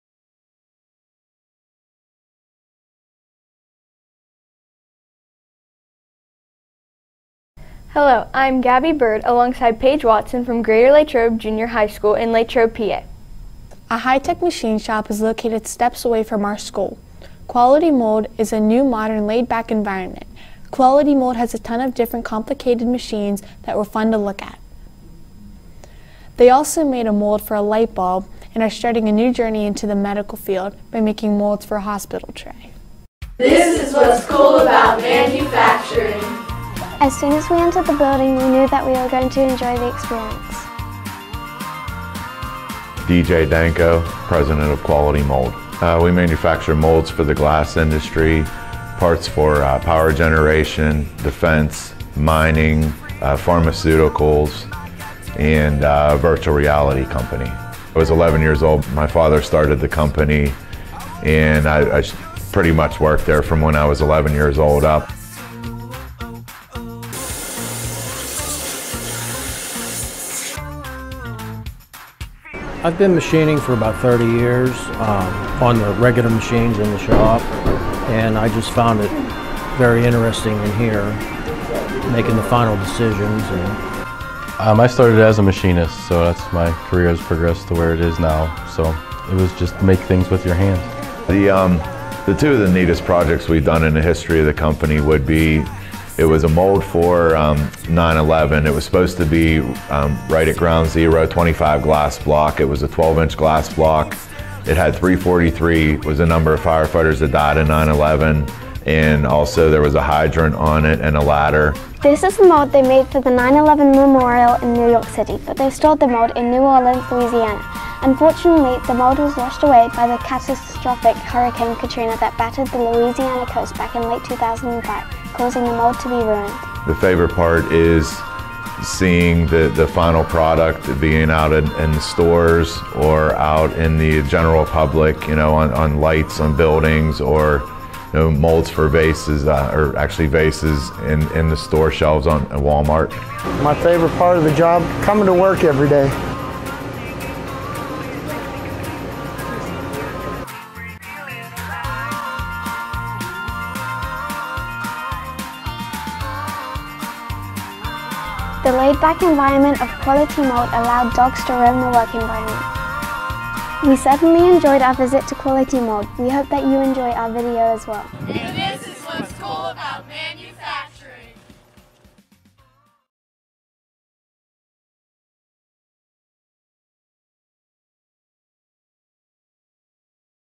2018 Student Video Contest - Greater Latrobe and Quality Mould
Greater Latrobe's submission for the Explore the New Manufacturing Student Video Contest at Quality Mould.

Schools from Southwestern Pennsylvania were partnered with regional manufacturing to create a video about “What Makes Manufacturing Cool?” To learn more about this program and other ways to Explore the New Manufacturing, visit us